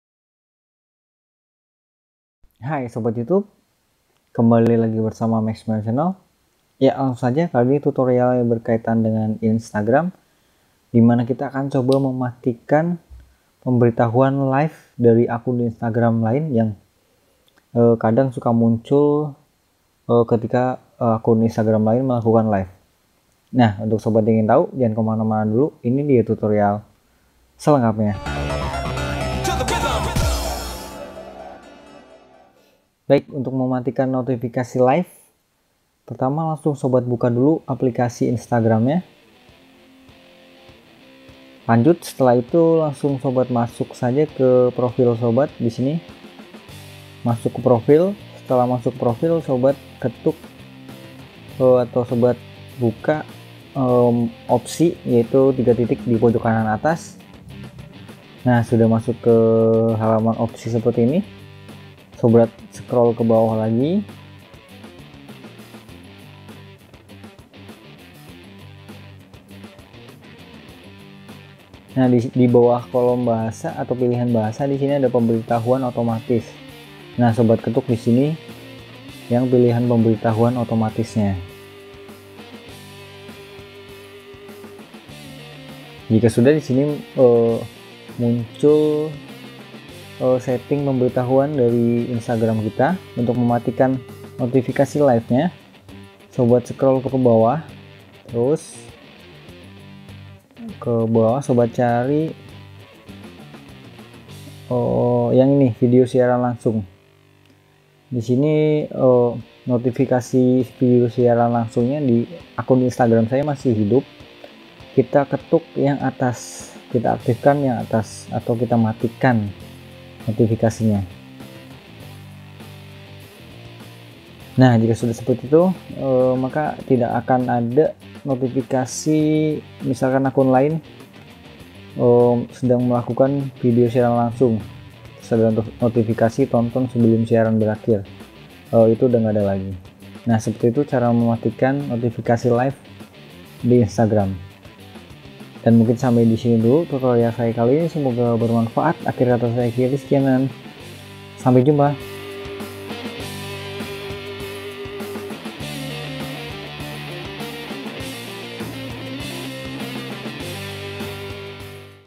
Tutorial Instagram - Cara mematikan notifikasi live akun instagram lain
Pasri kamu pernah mendapati pemberitahuan atau notofikasi dari aplikasi instagram bahwa sedang berlangsung sebuah live dari akun instagram yang kita follow atau ikuti.

Di Instagram memang mendukung fitur live seperti halnya facebook dan Youtube, jika sobat merasa terganggu dengan pemberitahuan live ini, sobat dapat matikanya dengan masuk ke menu setting atau pengaturan.

Untuk selengkapny silahkan simak video berikut.